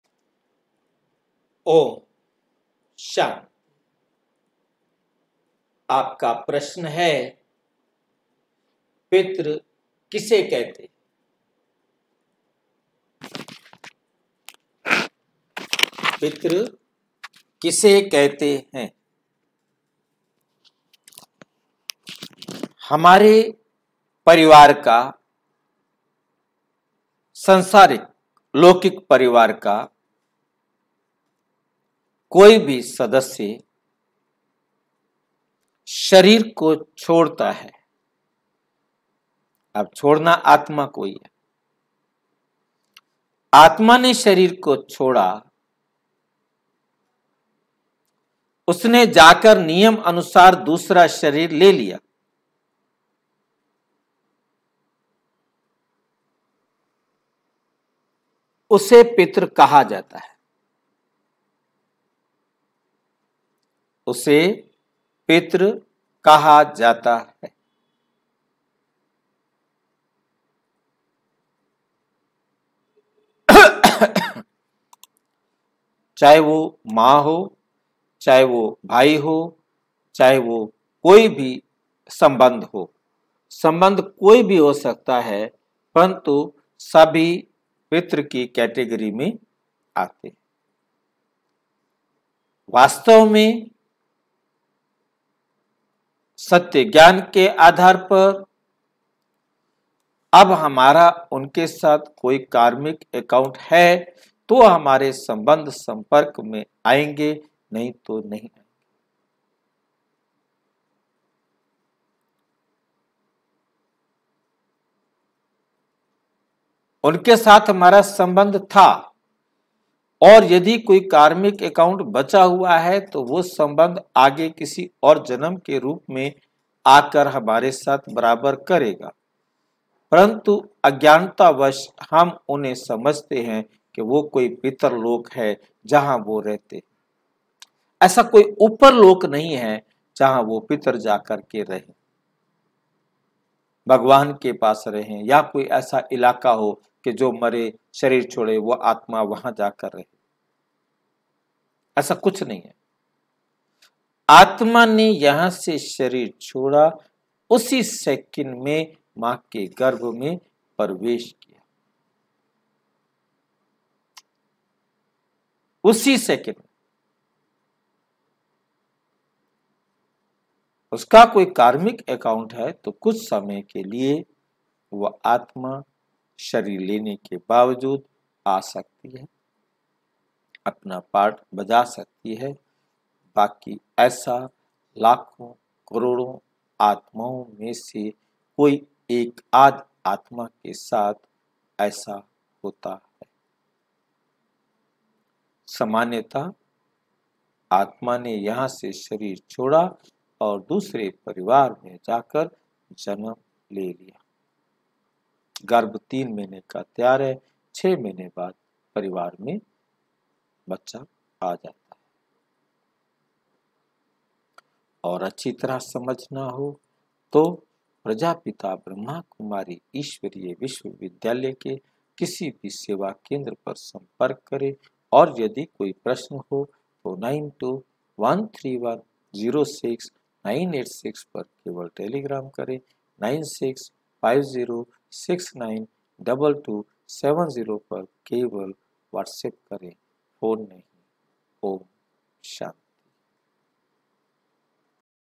पित्तर किसे कहते हैं?/Who are the fathers(pitar)?/BKDr Surender Sharma
The drama does not change, then how can we write elevated fortune by doing elevated deeds?
  Which children get the right to inherit the kingdom?
TODAY MURLI
BK MURLI
LIVE MURLI